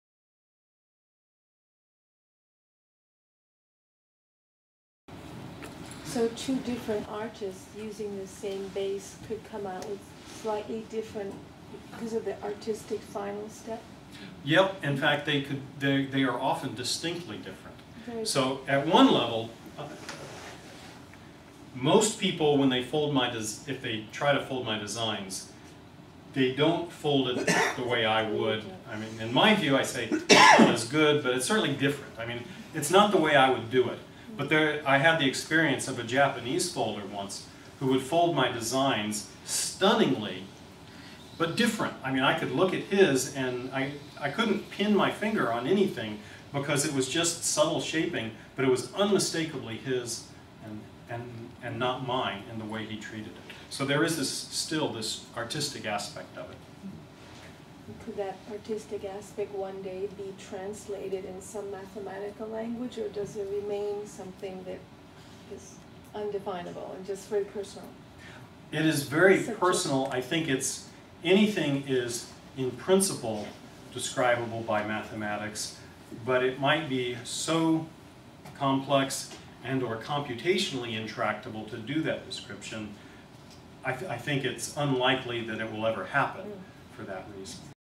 Robert Lang on Algorithms and Artistic Differences
Robert Lang discusses artistic differences in origami during his Distinguished Lecture at the MAA Carriage House on June 25, 2009.